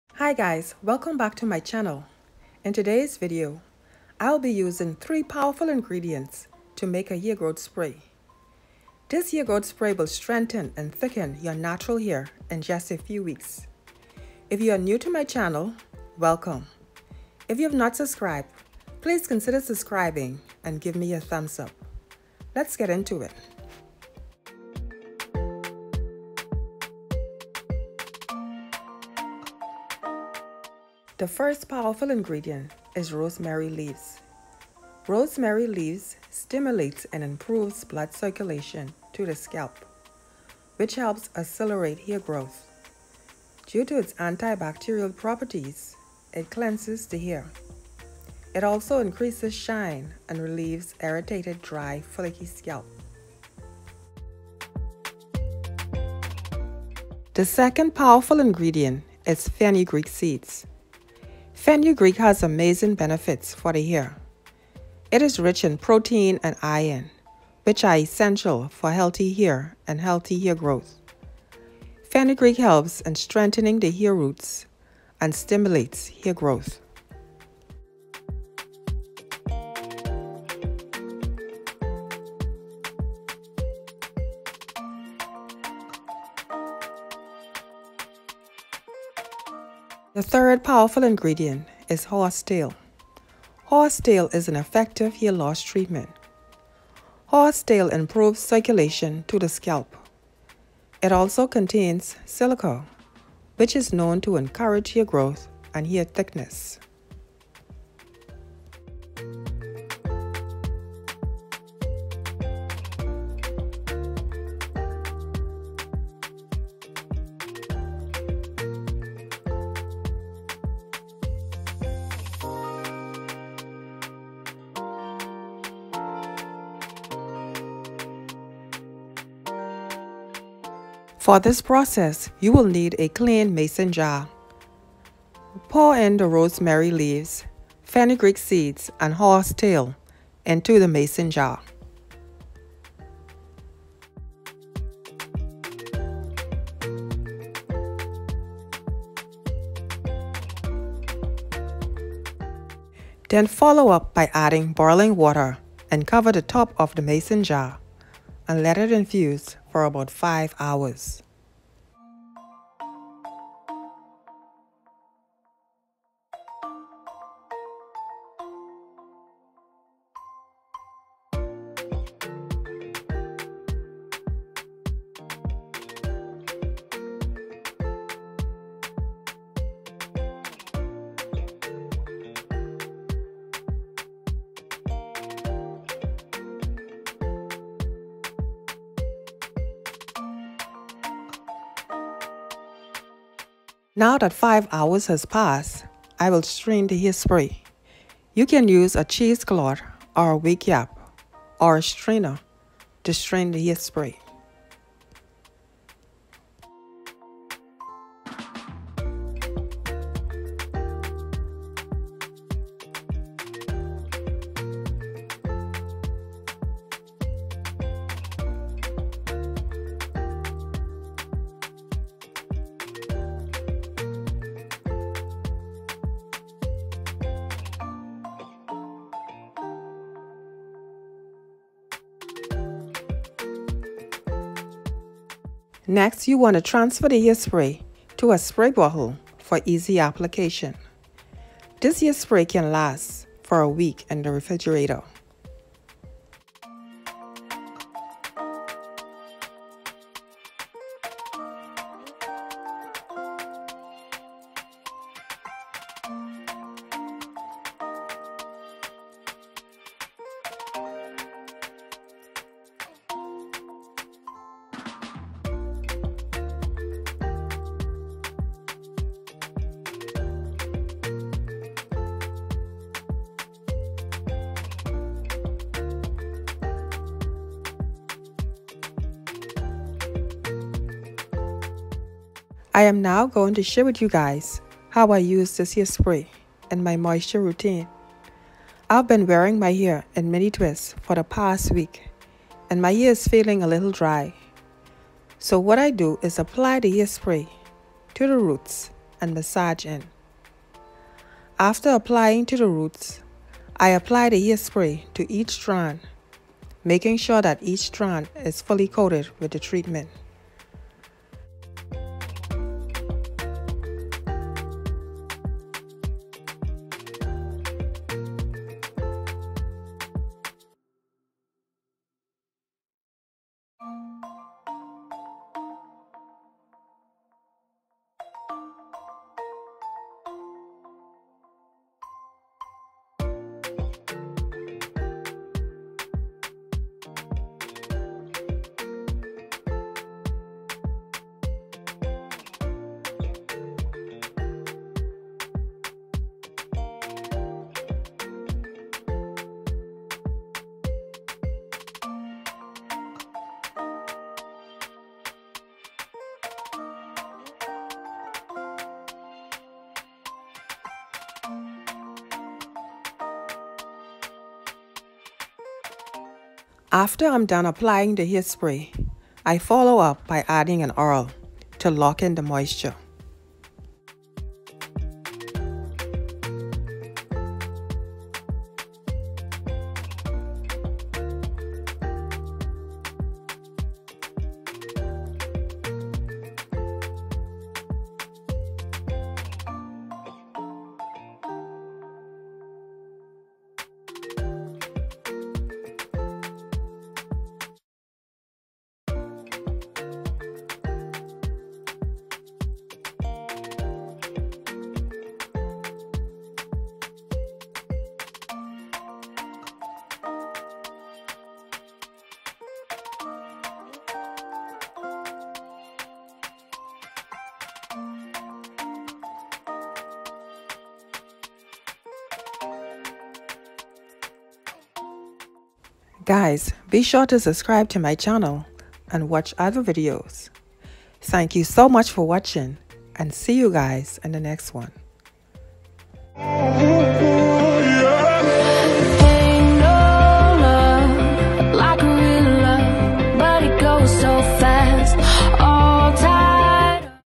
Combine these 3 Ingredients And Your Hair Will Grow Like Crazy || Do Not Wash It Out
Hi Beauties,
In today's video I share 3 Powerful Ingredients hair spray that will strengthen and thicken your natural hair.  The 3 powerful ingredients are (1)  Rosemary leaves (2) Fenugreek seeds (3) Horsetail all of which stimulate growth and healthy long natural hair.    This hair spray can applied to 3 times week and stored up to 7 days in the Refrigerator.

 Hope this video will be very informative and enjoyable
Thank you so much for being with us and supporting us

⏰Video Duration: 7:28